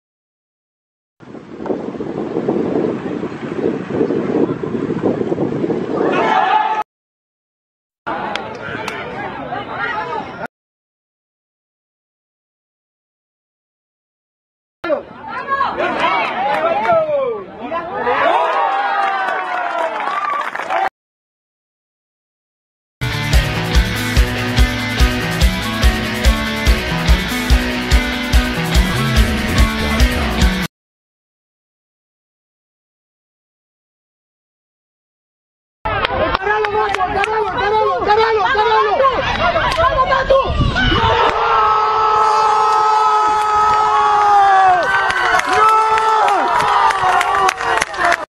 VÍDEO PRESENTACIÓN/ MATÍAS GÓMEZ
Matías Gómez/ Lateral Volante derecho/categoría 2002/ jugador de San Lorenzo de Almagro.